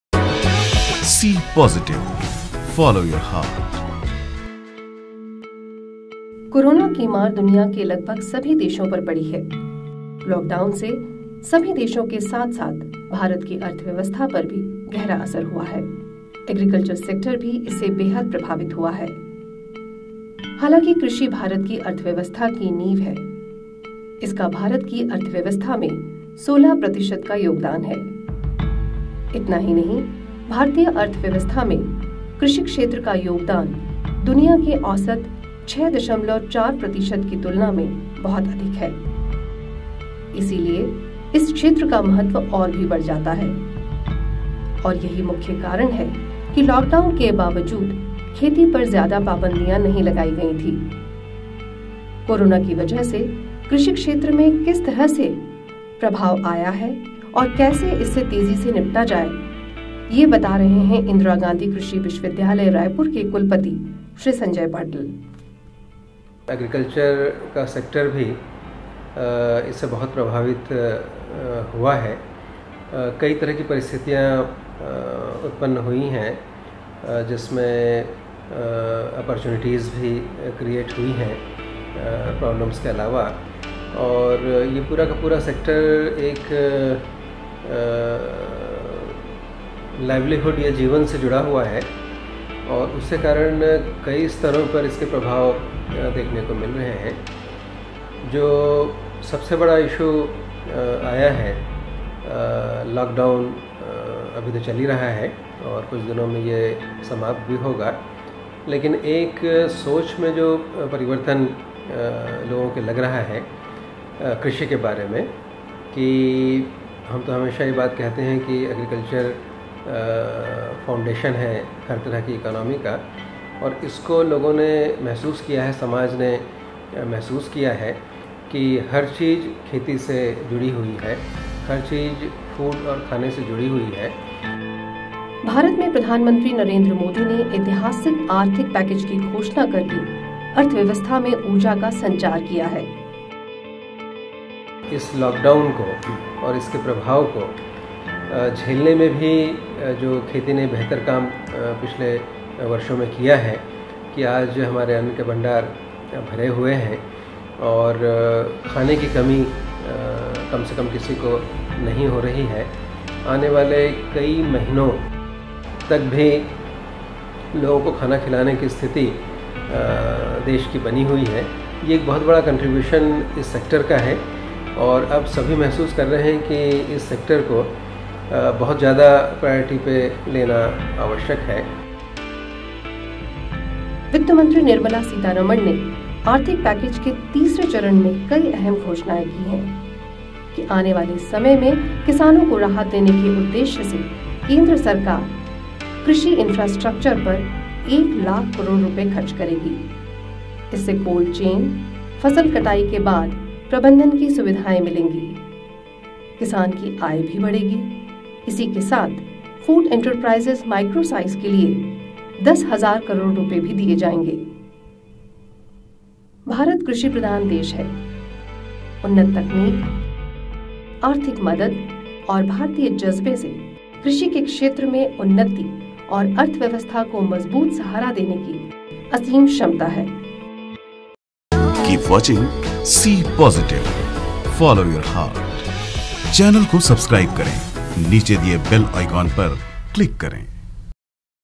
Agriculture Sector and new avenues: IGKV
In this video, Mr. Sanjay Patil, VC of Indira Gandhi Krishi Viswavidyalaya speaks about the present condition of agriculture sector in India and how will it recover post Corona.